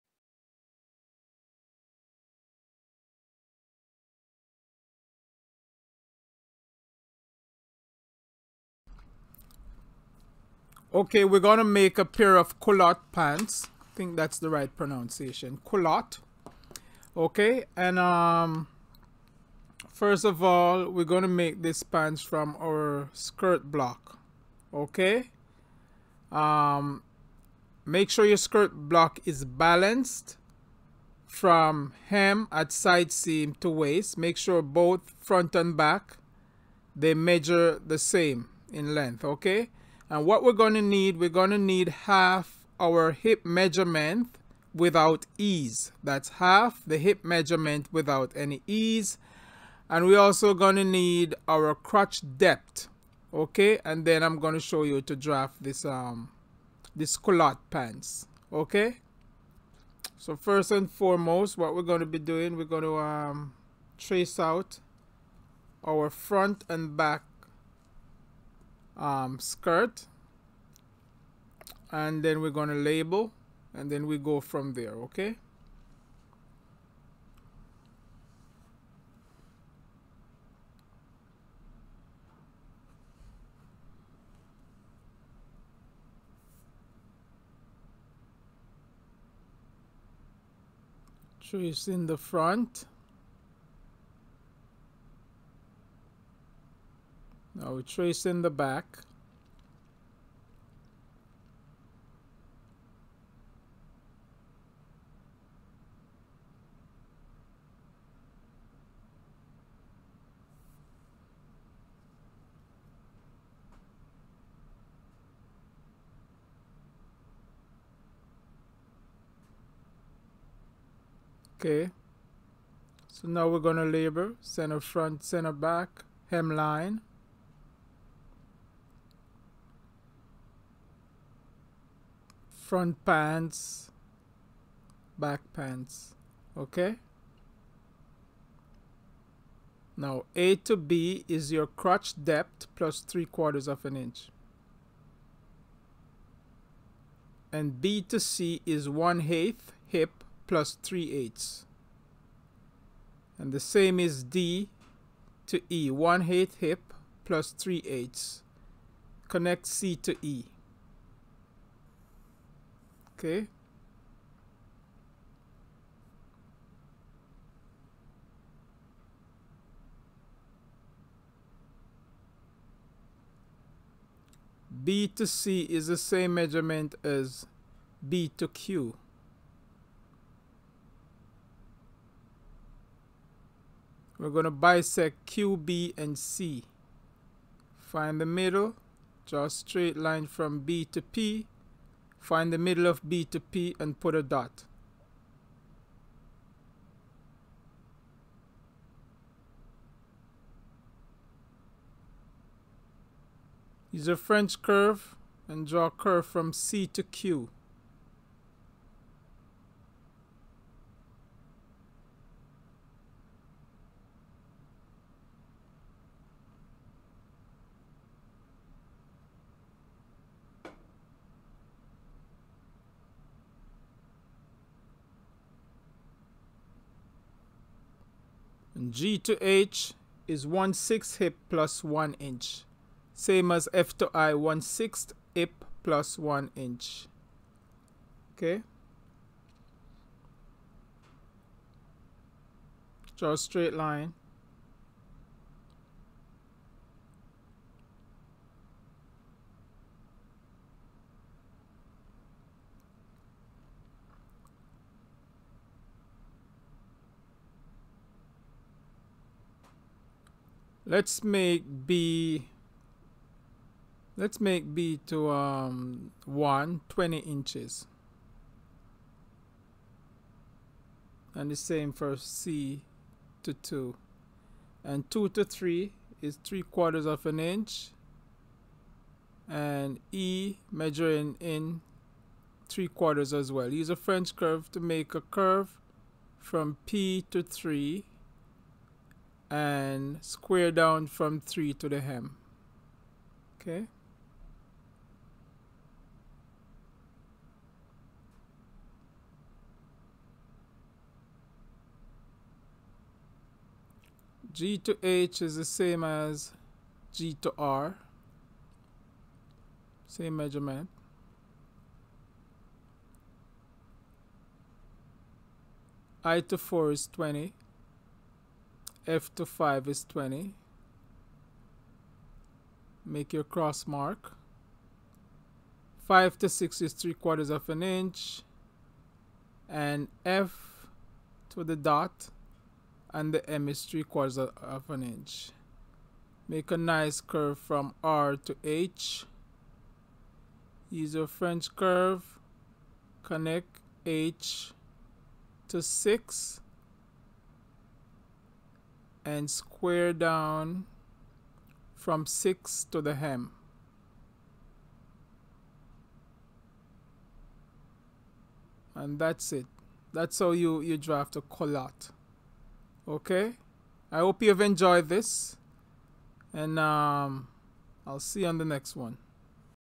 Culotte Pants for Beginners
This video gives instructions on how to draft a basic Culotte. It is simple and easy enough for beginners. I have not done any slash and spread as this video is aimed at beginners.